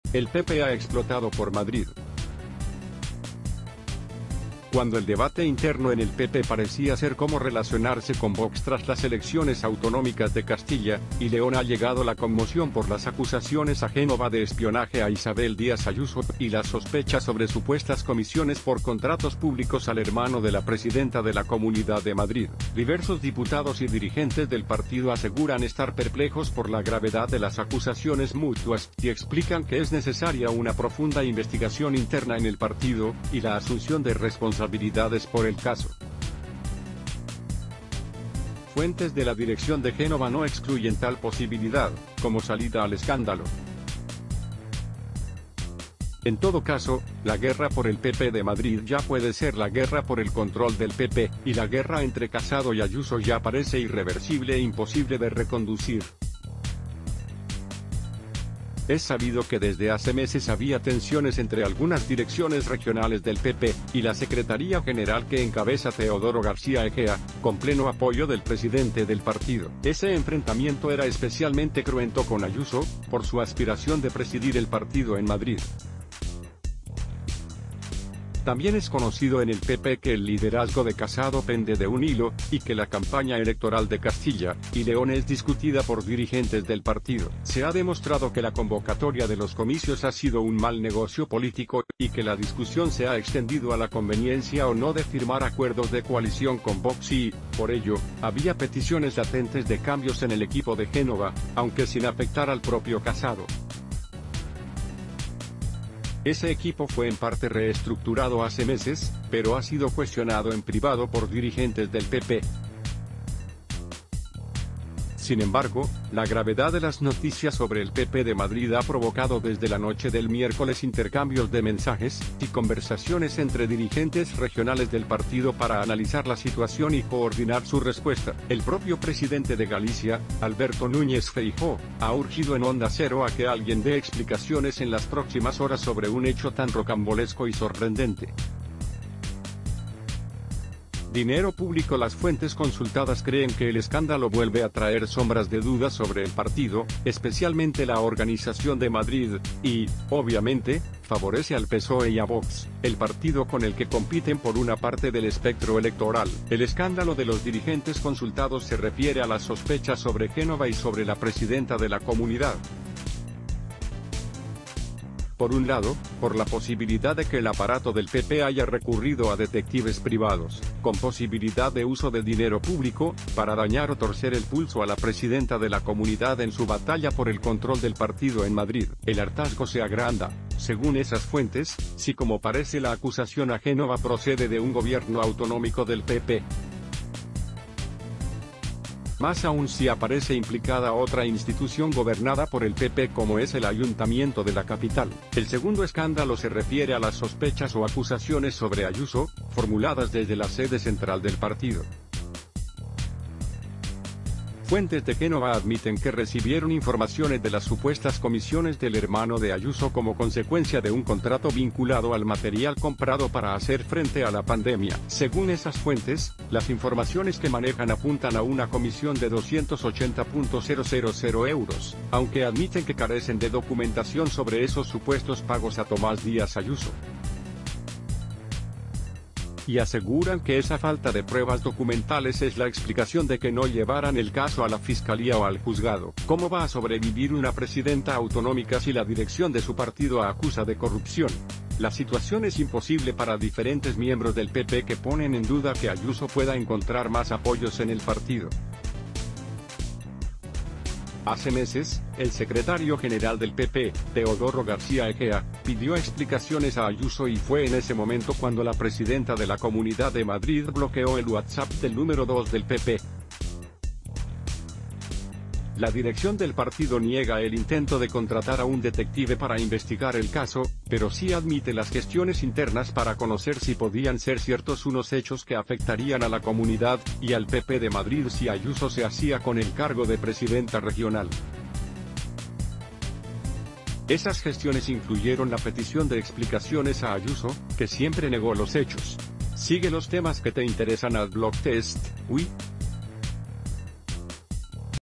Dirigentes del PP piden una investigación interna y responsabilidades por el doble escándalo de...
Dirigentes del PP piden una investigación interna y responsabilidades por el doble escándalo de Madrid

El PP ha explotado por Madrid.